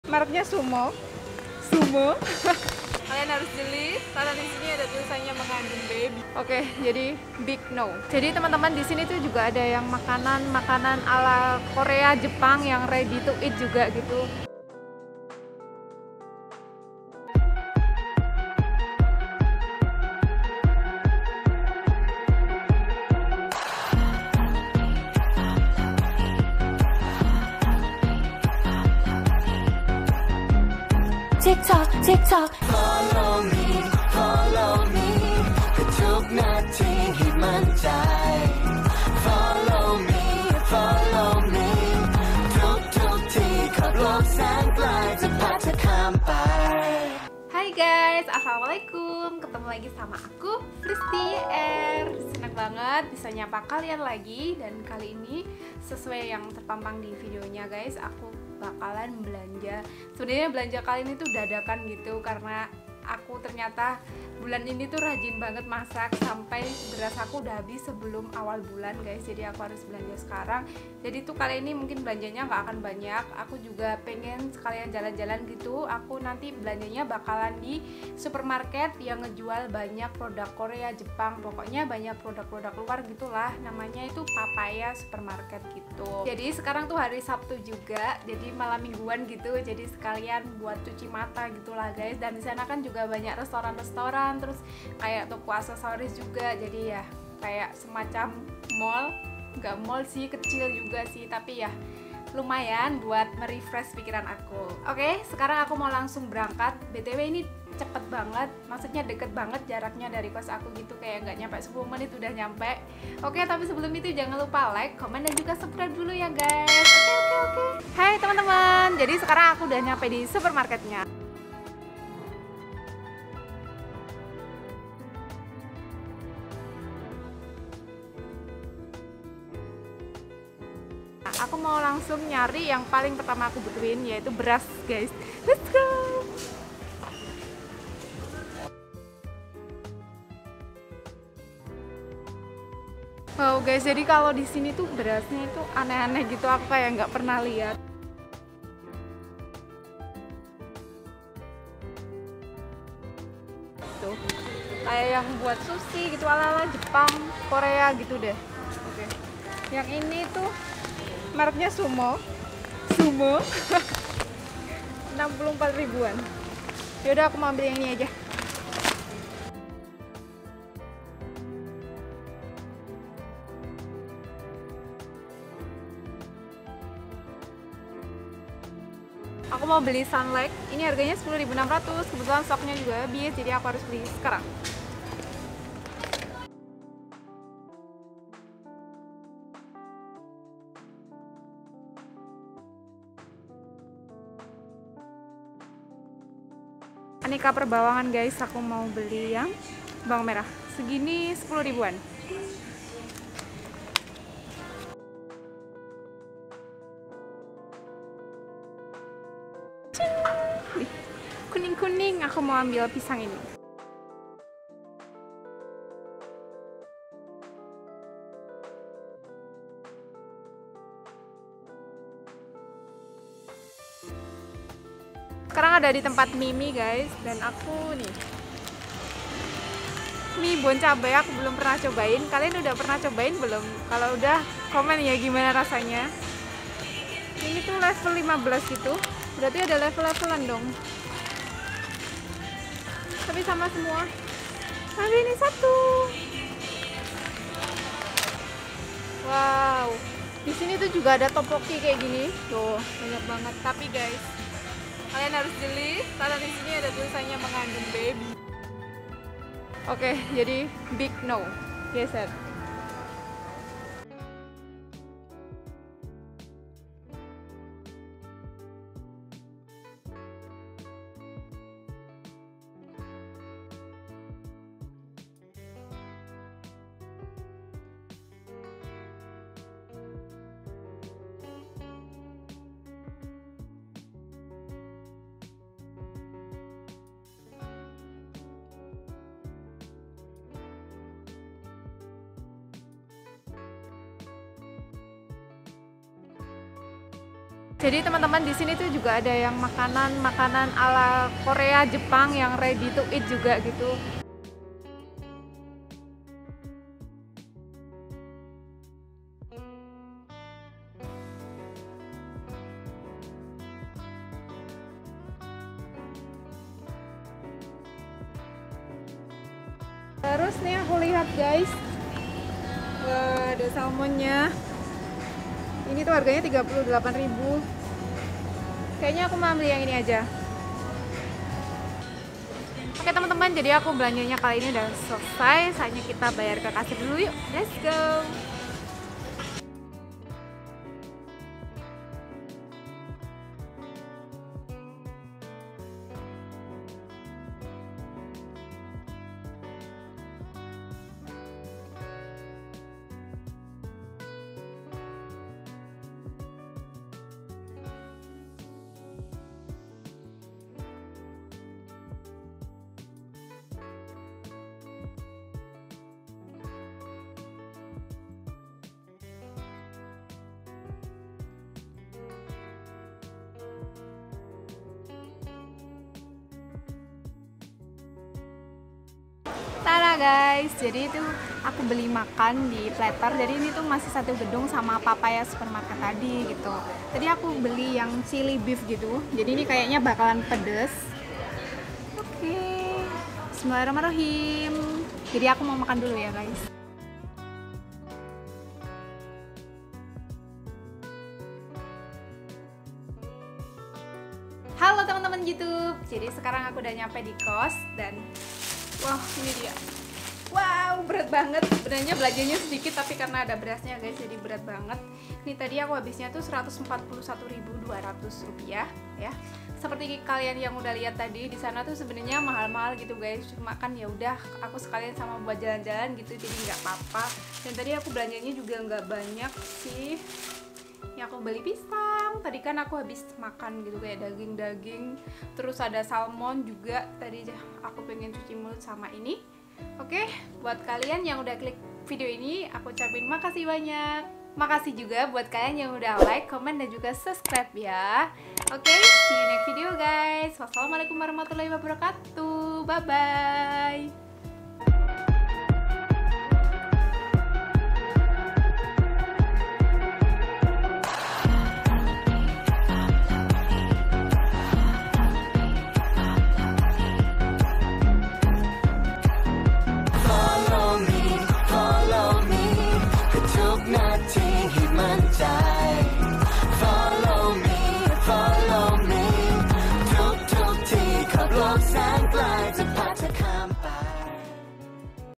BELANJA DADAKAN DI PAPAYA FRESHMARKET | Banyak produk Korea + Jepang !
Assalamualaikum temen2 youtube !

Di video kali ini aku belanja dadakan di papaya freshmarket. Happy banget disini banyak produk korea + jepang. Cuci mata mumpung lg malam minggu heheh

I hope you enjoy and happy watching 😄 .